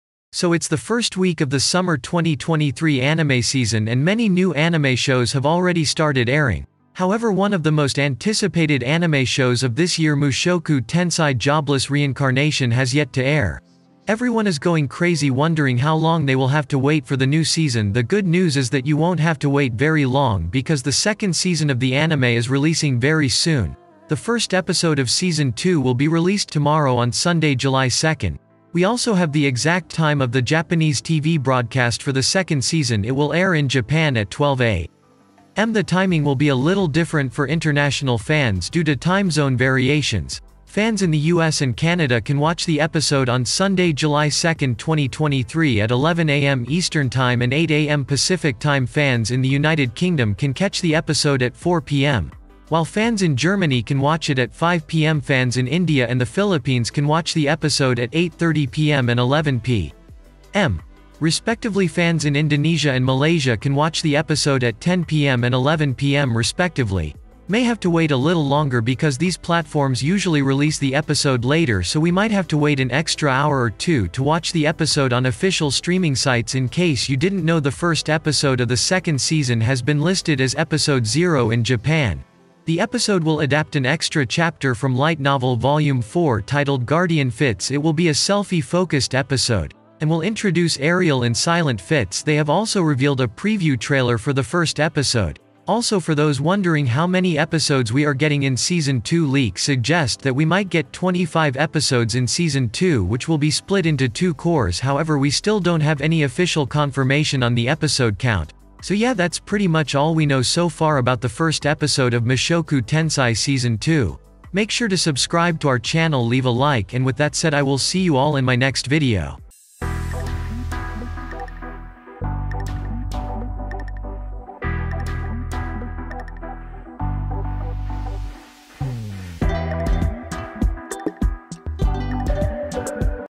Mushoku Tensei Season 2 Episode 1 Release Date and Time
Mushoku Tensei Season 2 Episode 1 Release Date, Release Time, Episode Count Leaks, and More Info.
Mushoku Tensei Ⅱ: Isekai Ittara Honki Dasu,
Mushoku Tensei: Jobless Reincarnation Season 2,
mushoku tensei season 2 episode 0